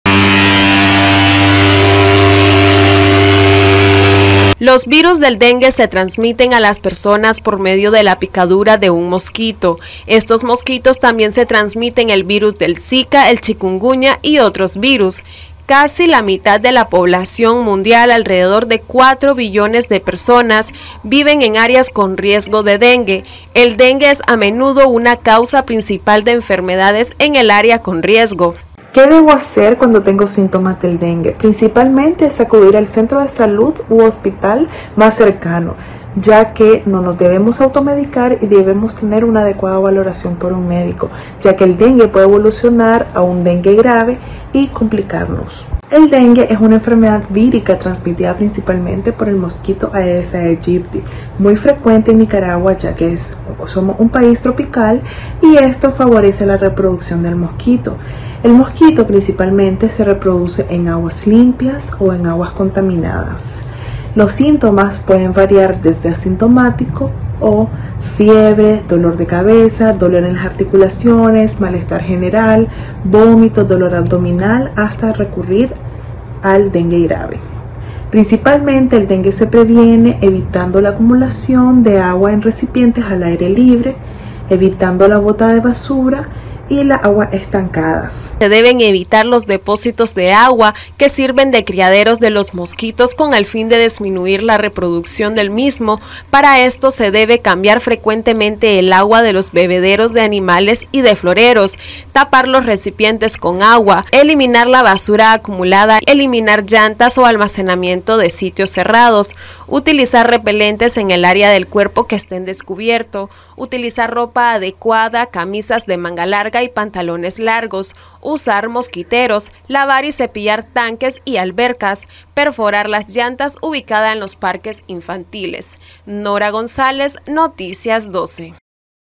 Juntos y juntas podemos prevenir el dengue
Dra. castillo reitera a la población la importancia de mantener las medidas de prevención contra el Dengue